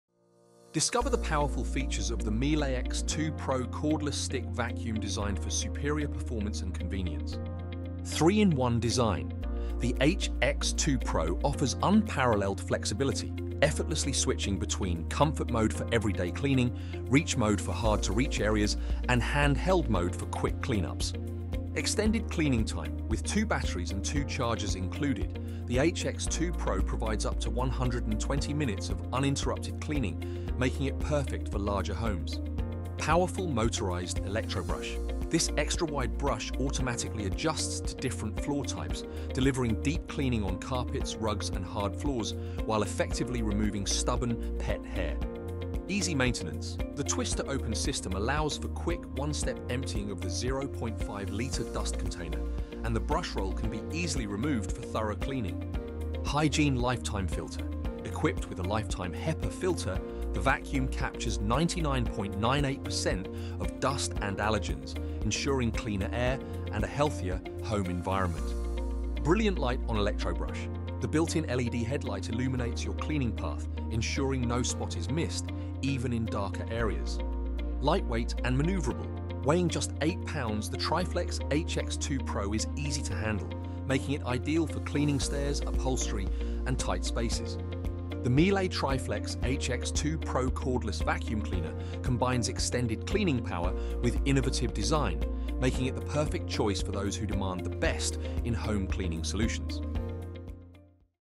Miele Triflex HX2 Pro Stick Vacuum Features - Superior Vacuums
“Discover the standout features of the Miele HX2 Pro Triflex – from its versatile 3-in-1 design to powerful suction and dual batteries, this stick cordless vacuum is built for ultimate convenience and performance.”